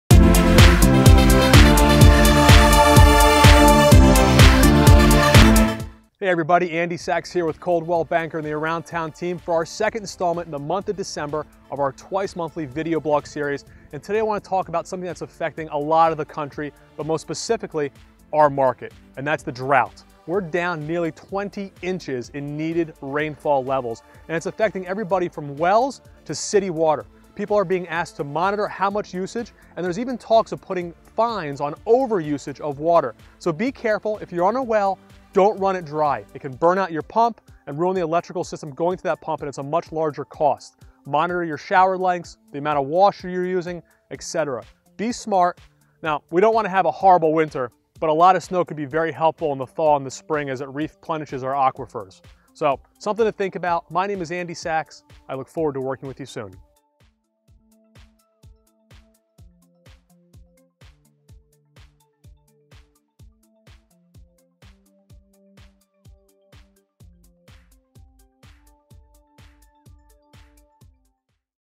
Connecticut Real Estate Agent: How to Combat Our Current Drought
We are in the midst of a tough drought right now. We are nearly 20 inches down in needed rainfall levels and it’s affecting everyone. There have even been talks of a potential fine for over usage of water. We can each do our part to help conserve water for the time being. To learn some easy tips that you can use to reduce your consumption, watch this short video.

Andy Sachs
The Around Town Team
Newtown, Connecticut

TRANSCRIPTION:

I wanted to talk about something today that’s affecting a lot of the country, but especially our market—the drought. We are down nearly 20 inches in needed rainfall levels and it’s affecting everyone from the wells to the city water. People are being asked to monitor their usage, and there are even talks of putting a fine on overuse of water.

So be careful. If you’re on a well, don’t run it dry. It can burn out your pump and ruin the electrical system going to that pump, which is a much larger cost.

Monitor your shower lengths, as well as the amount of water you use when washing your hands or your dishes.

We don’t want to have a horrible winter, but a lot of snow could be helpful to replenish our aquifers at this time. It’s something to think about.